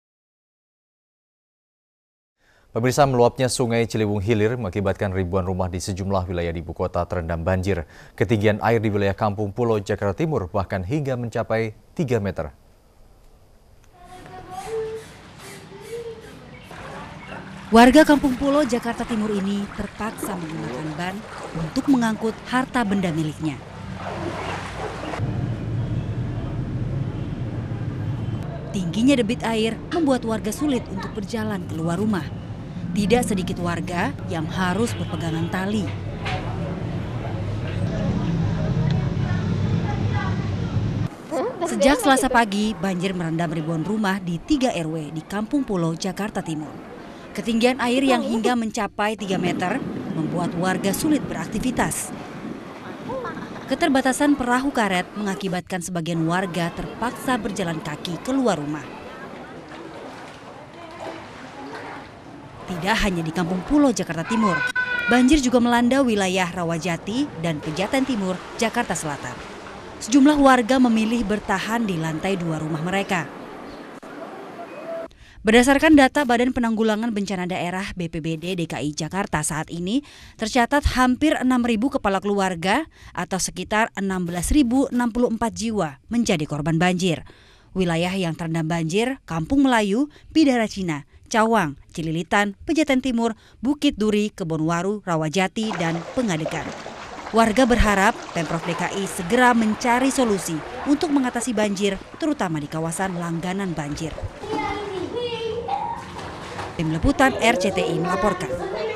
Ketinggian Air di Kampung Pulo Capai 3 Meter
Meluapnya sungai Ciliwung hilir menyebabkan ribuan rumah terendam banjir.